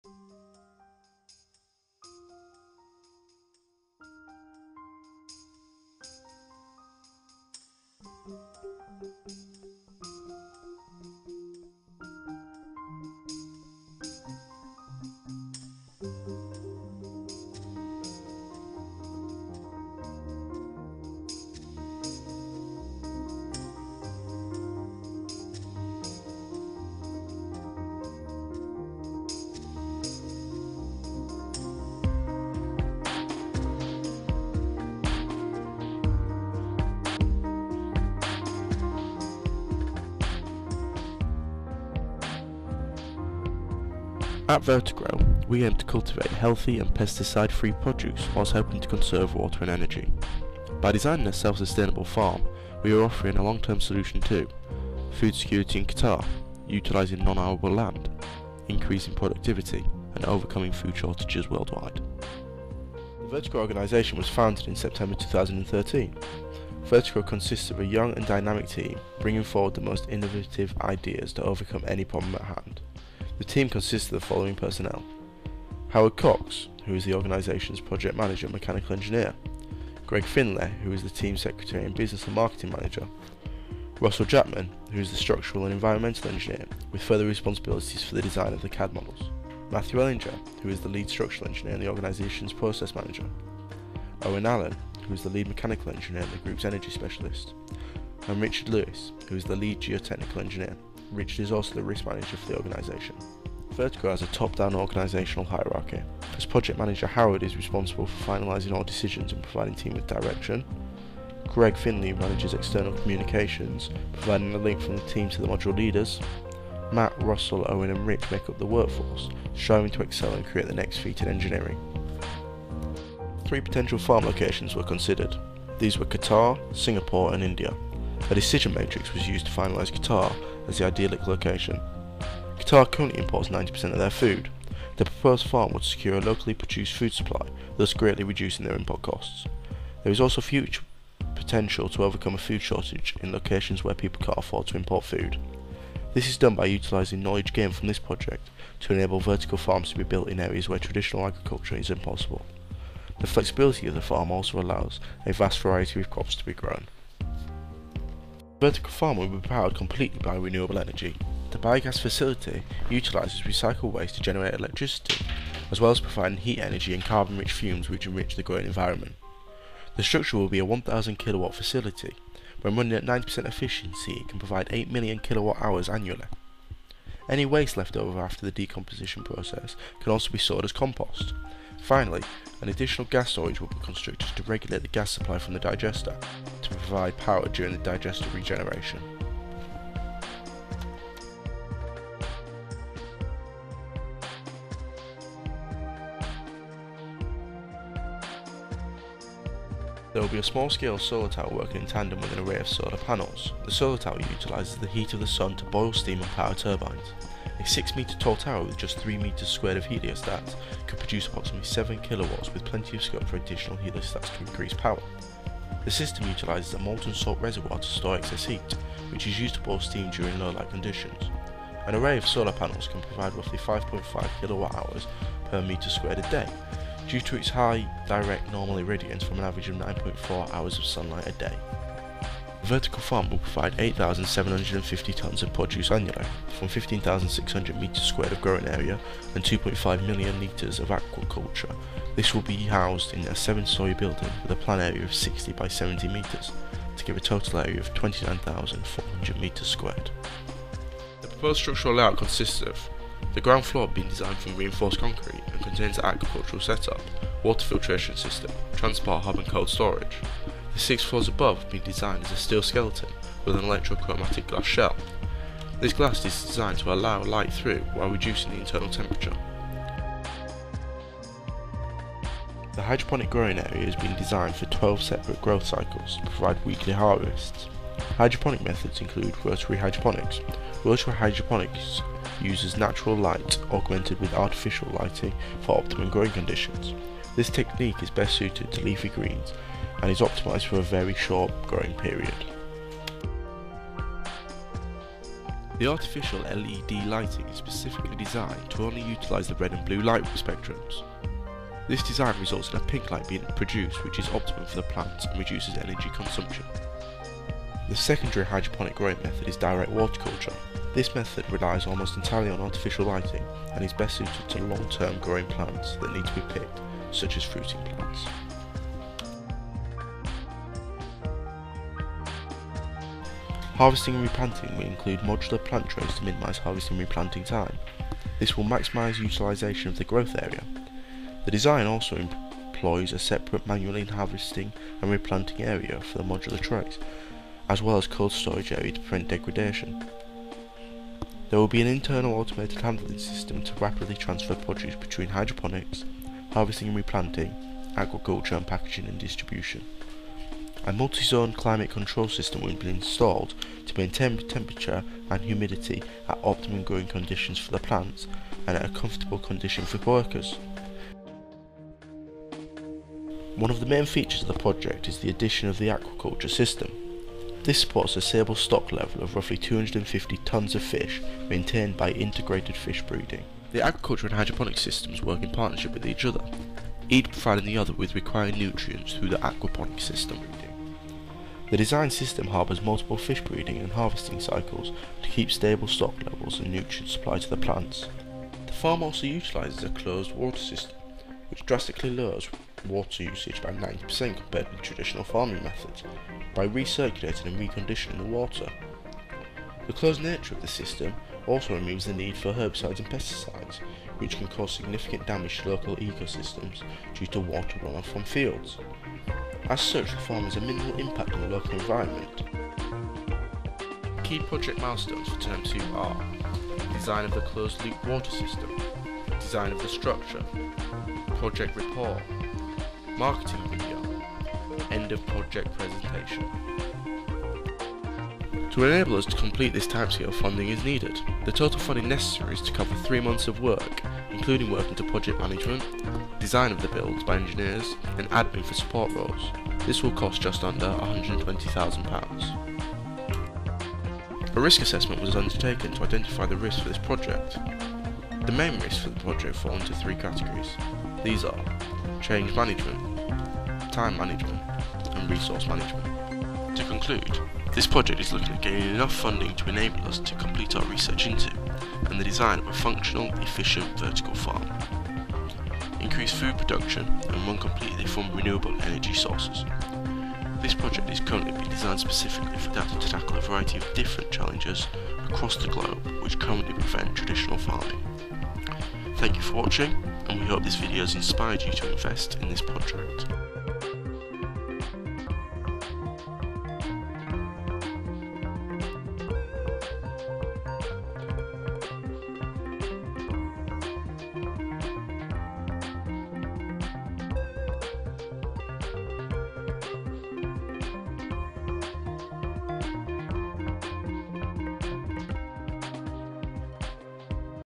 VertiGrow Design Concept
If you like this video please watch the the marketing video which covers this project in more detail and better quality, with an improved voice over, which can be found

The design concept video for a vertical farm based in Qatar created by MEng Civil Engineering & MEng Mechanical Engineering students as part of the final year Interdisciplinary Design Project at the University of Plymouth.

With no prior experience in video production or editing, all the skills required to create this video were learnt over the course of just three weeks between between working on other assignments.